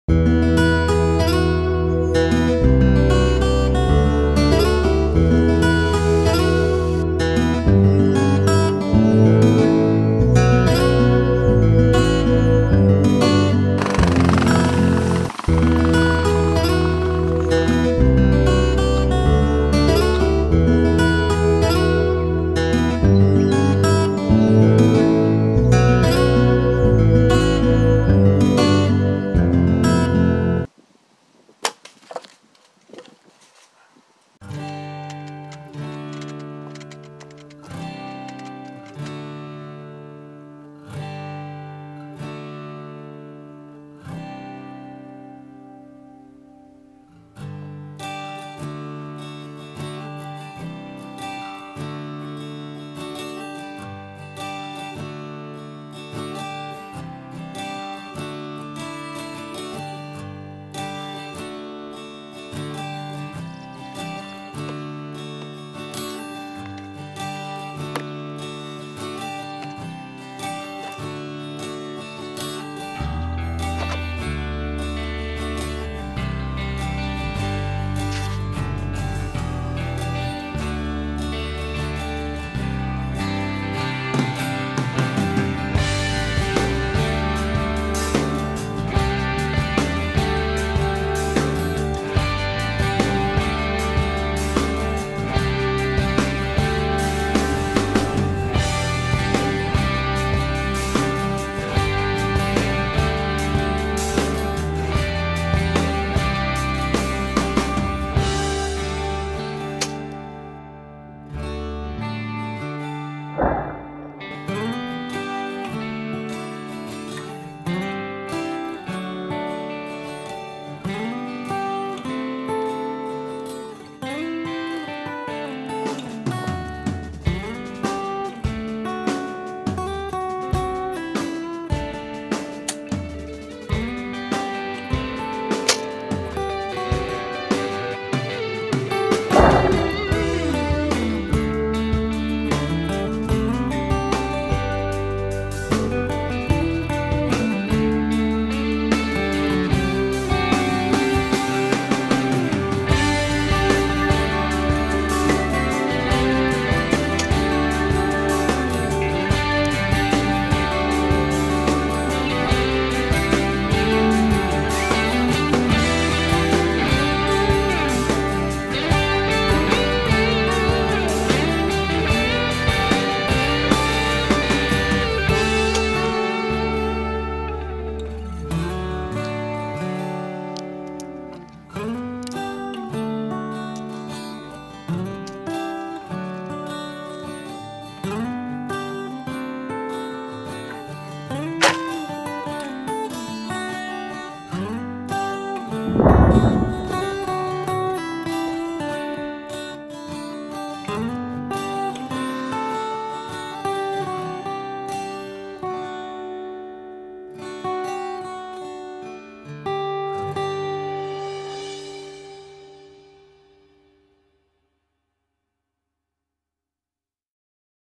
Bow Shooting Practice
In this video I am shooting my infinite edge pro bow from diamond archery. It is a really nice bow for treestand hunting.